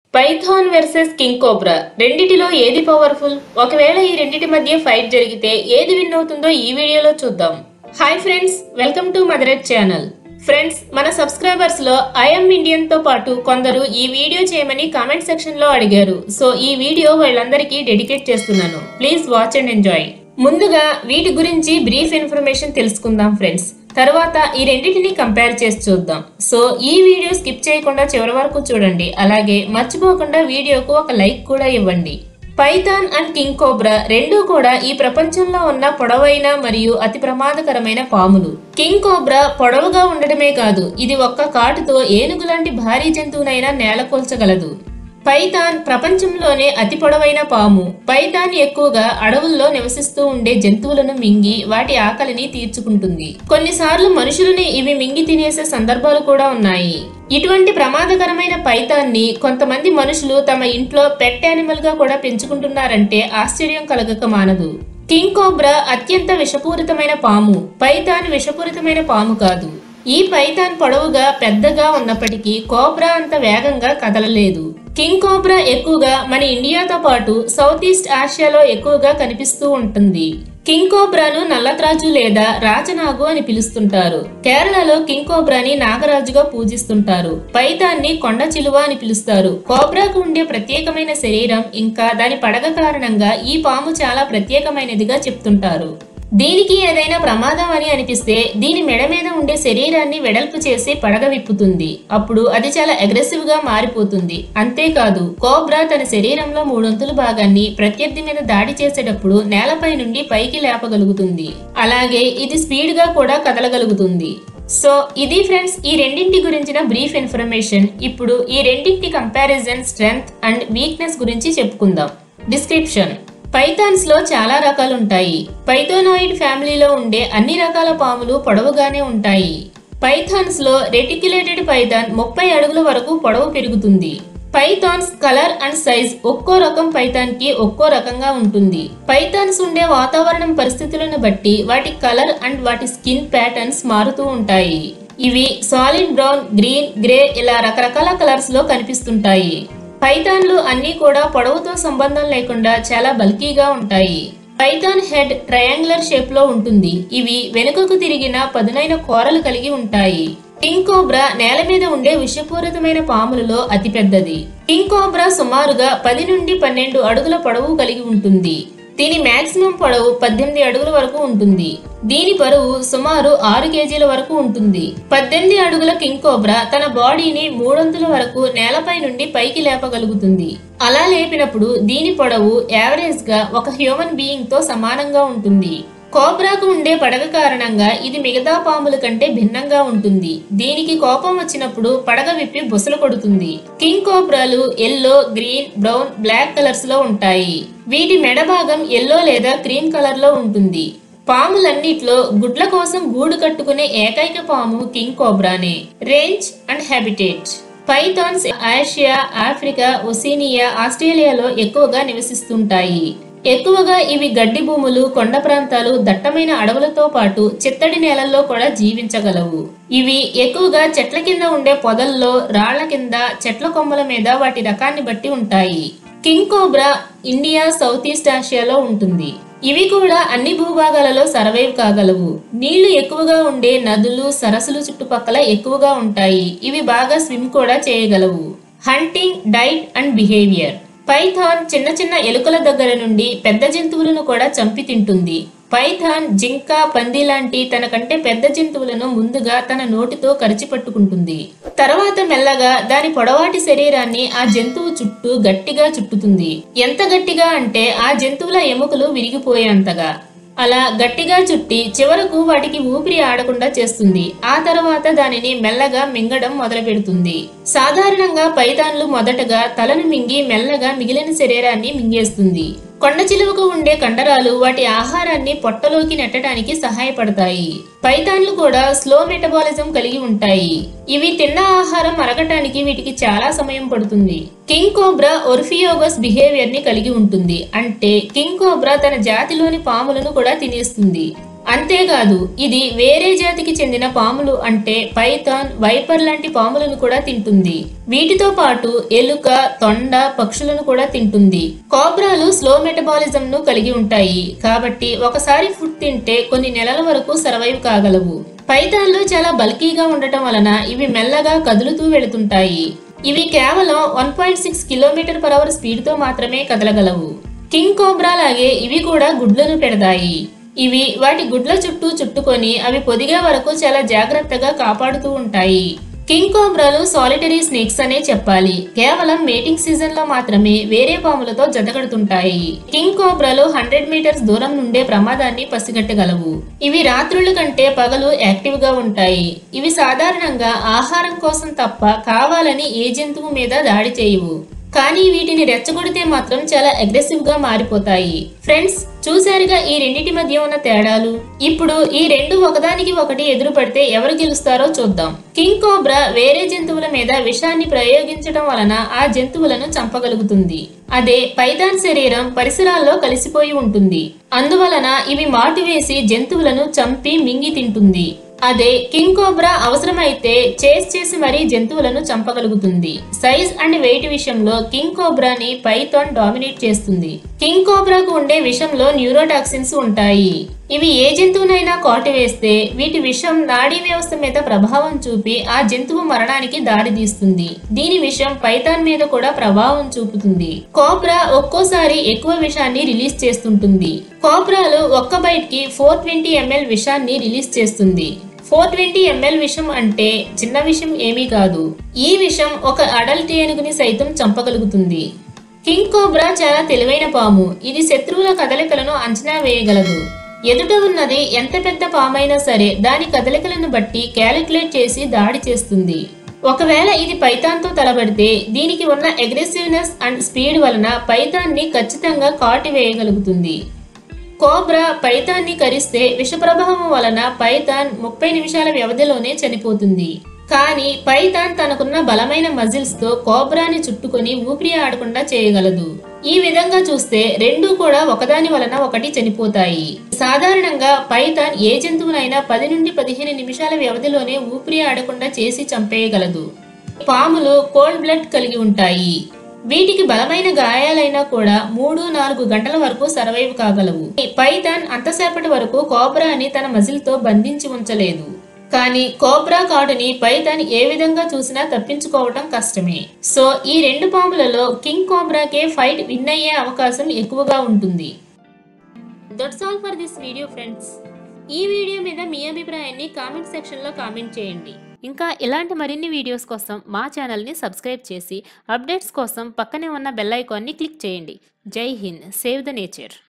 PYTHON VS KING COBRA - WHO WILL WIN A FIGHT IN TELUGU | Mother Earth Channel
Topics covered in this video:
PYTHON VS KING COBRA - WHO WILL WIN A FIGHT IN TELUGU
king cobra vs python - who wins the fight?

about my channel:
Mother Earth Channel has natures, animals’ birds, wildlife videos, natural scenery,  etc. that can help you to get relax and having peaceful life.

We do not claim these clips as our own.